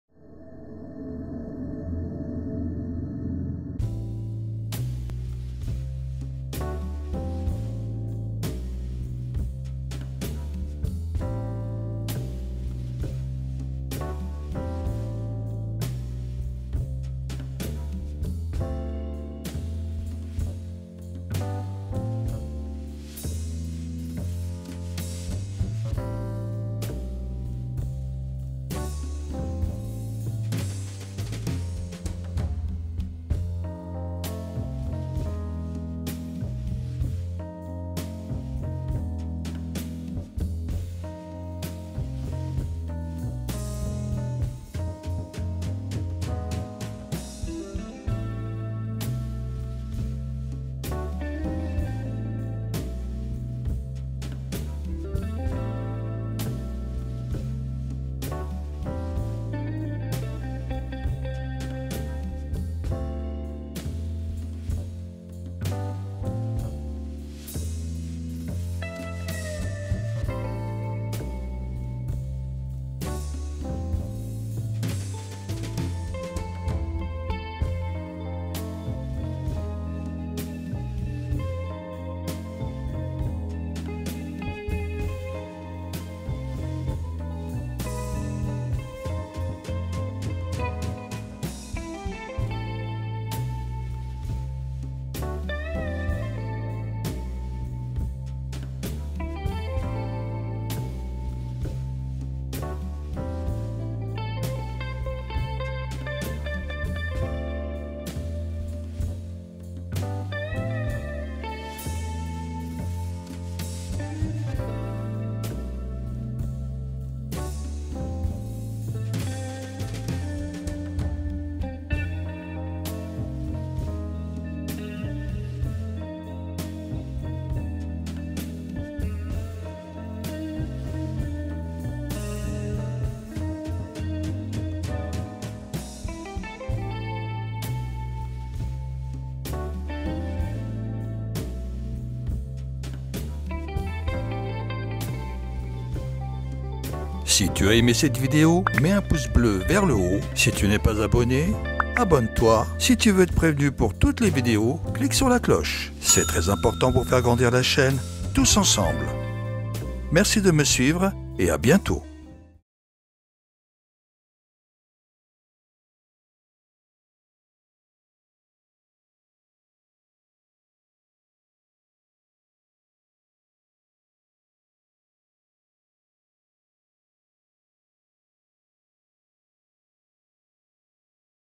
Comment télécharger Gimp retoucheur de photos pour MACOSX
Gimp est un logiciel de retouche photos  et d'édition. Cette version est pour